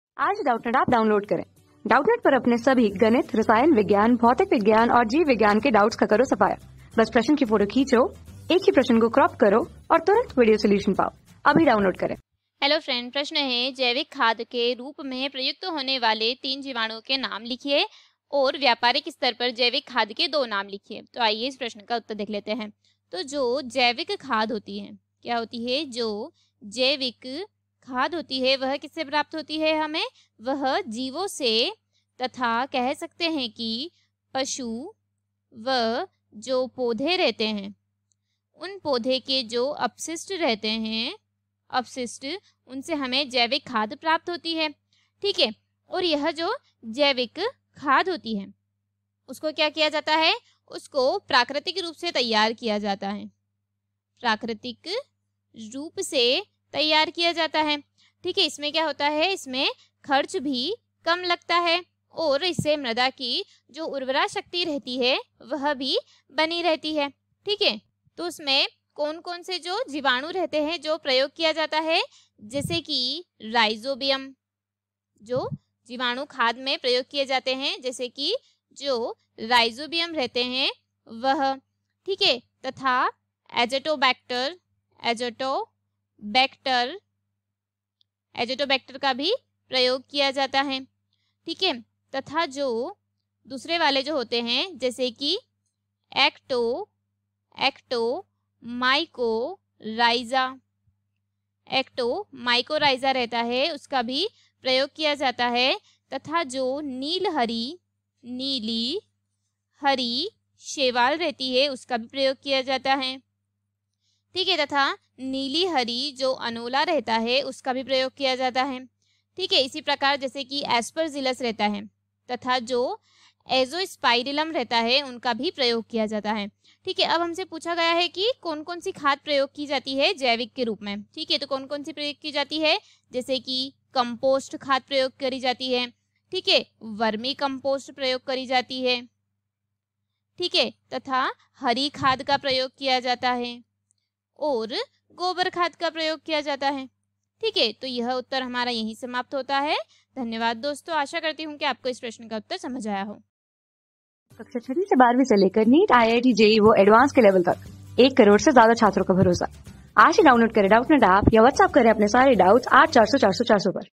जैविक खाद के रूप में प्रयुक्त होने वाले तीन जीवाणुओं के नाम लिखिए। व्यापारिक स्तर पर जैविक खाद क...
जैविक खाद के रूप में प्रयुक्त होने वाले तीन जीवाणुओं के नाम लिखिए। व्यापारिक स्तर पर जैविक खाद के दो नाम लिखिए।
Class: 12
Subject: BIOLOGY
Chapter: मानव कल्याण में सूक्ष्मजीव
Board:Madhya Pradesh Board

👉 Have Any Query? Ask Us.